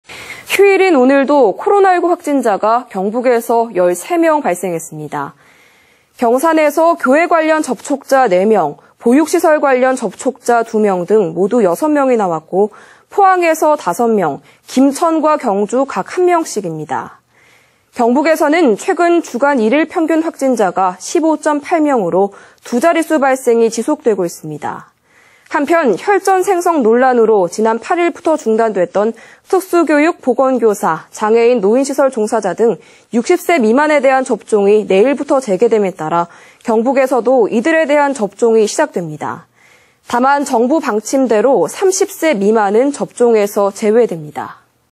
휴일에도 경북 확진 13명 발생 / 안동MBC
2021/04/11 18:05:47    작성자 : 이정희

휴일인 오늘도 코로나19 확진자가
경북에서 13명 발생했습니다.
◀ＥＮＤ▶

경산에서 교회 관련 접촉자 4명, 보육시설 관련 접촉자 2명 등 모두 6명이 나왔고
포항에서 5명, 김천과 경주 각 한 명씩입니다.

경북에서는 최근 주간 일일 평균 확진자가
15.8명으로 두 자릿수 발생이 지속하고
있습니다.

한편 '혈전' 생성 논란으로 지난 8일부터
중단됐던 특수교육·보건교사, 장애인·노인
시설 종사자 등 60세 미만에 대한 접종이
내일(12)부터 재개됨에 따라
경북에서도 이들에 대한 접종이 시작됩니다.

다만 정부 방침대로 30살 미만은 접종에서
제외됩니다.